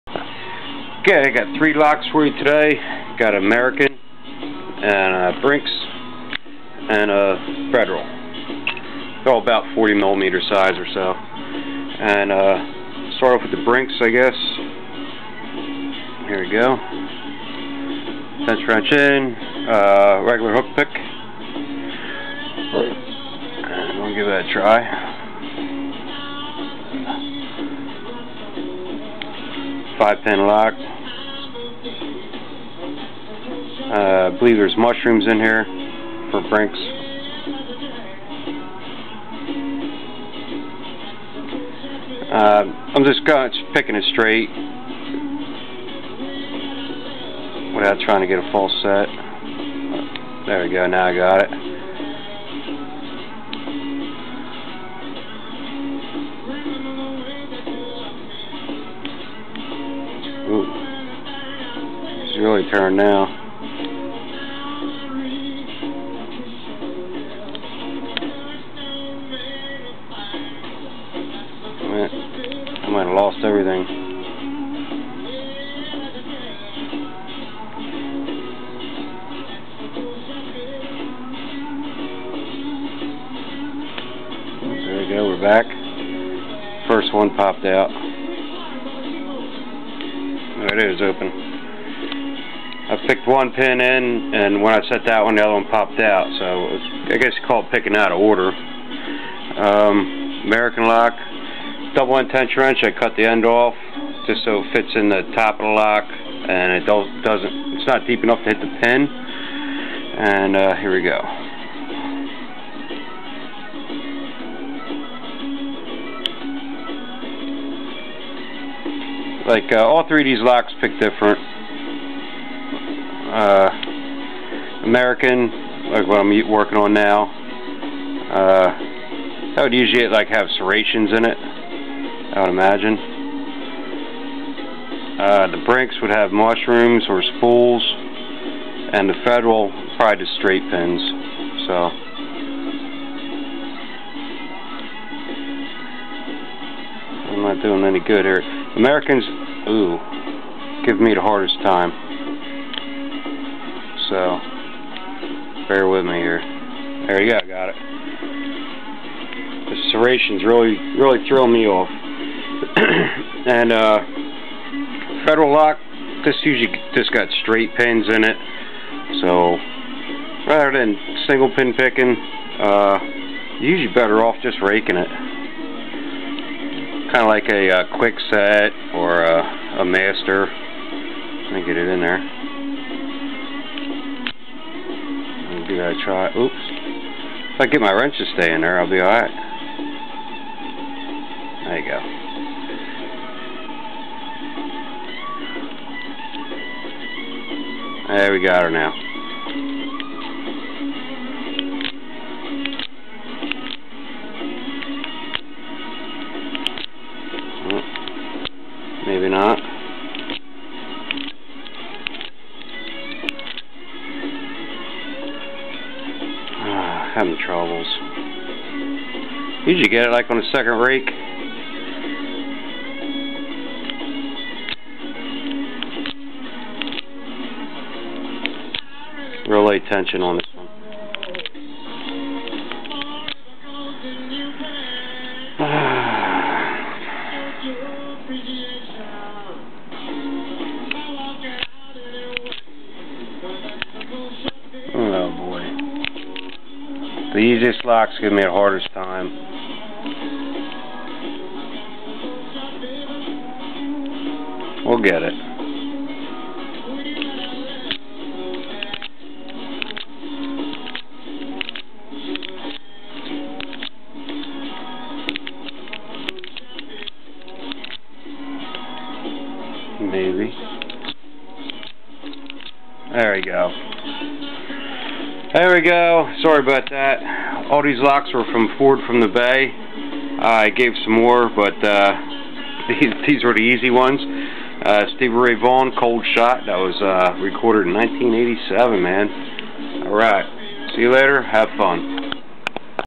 Got the Raking Blues
Brinks and American SPPed, Federal poorly raked.
I messed up on purpose so you could hear SRV jam a little longer!  ;¬)